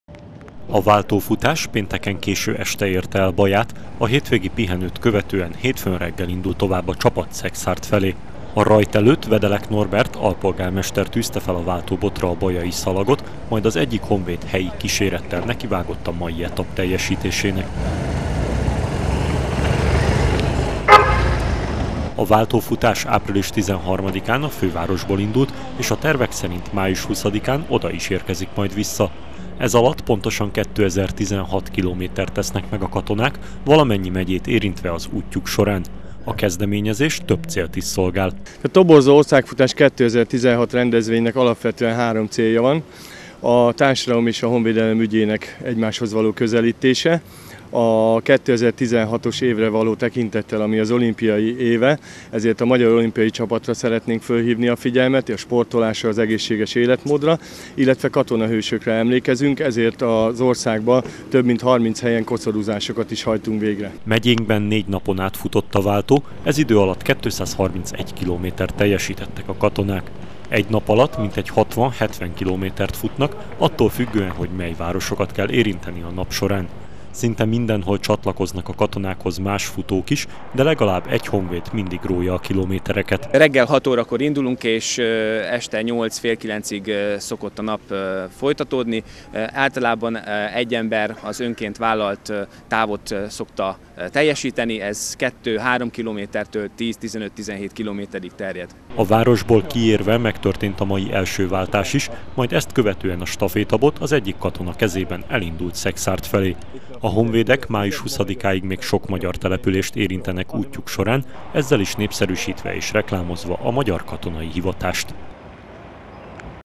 Népszerűsítő futás
Katonák jelentek meg hétfőn reggel a Szentháromság téren. Ezúttal azonban nem a migrációs válság vagy egy hadgyakorlat miatt jöttek Bajára, hanem egy országos váltófutás részeként érintették városunkat. A hadsereg emberei több dologra szeretnék felhívni a figyelmet a bő egy hónapos útjuk során.